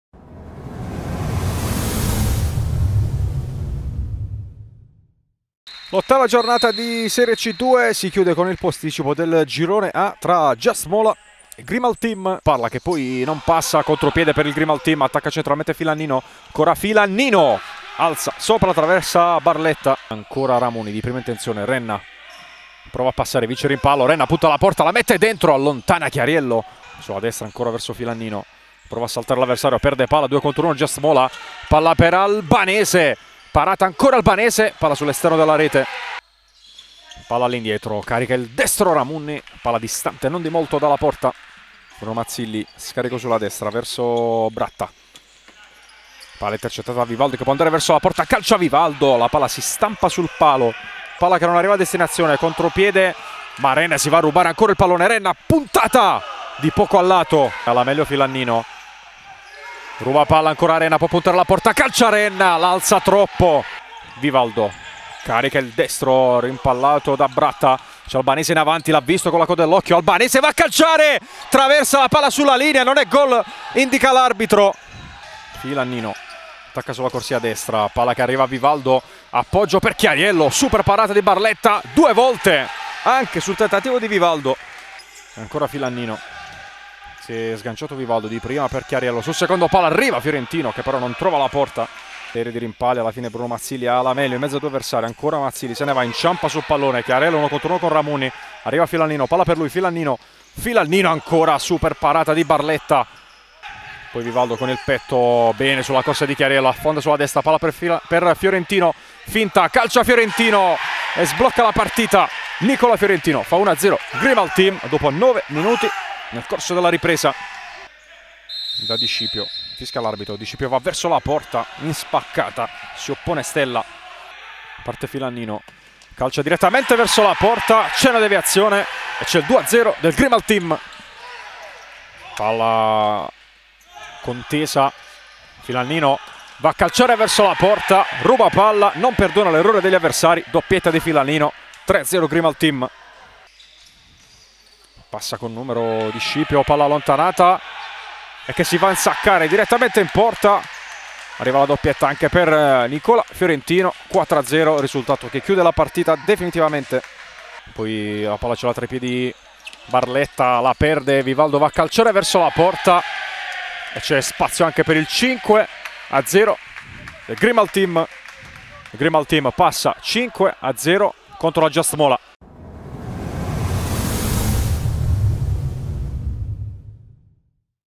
Just Mola-Grimal Team 0-5 (highlights)
Serie C2 girone A 2021-22, 8a giornata
Immagini Just Mola
© Puglia5